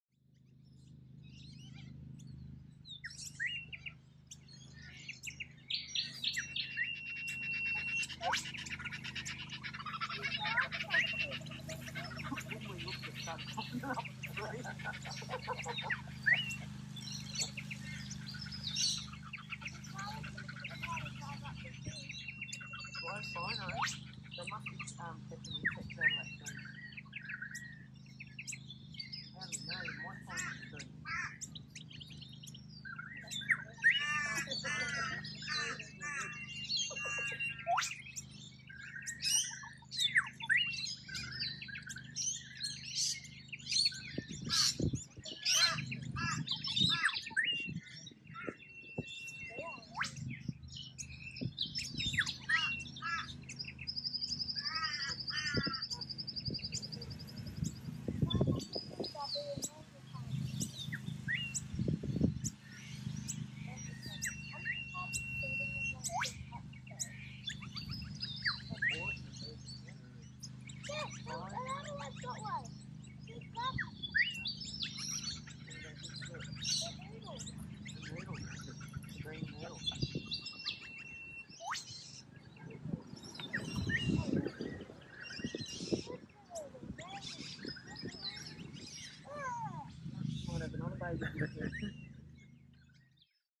Baby Miner Bird Falls from Nest yet Mum & Dad love's embrace never fades.
BRAVO AUSTRALIA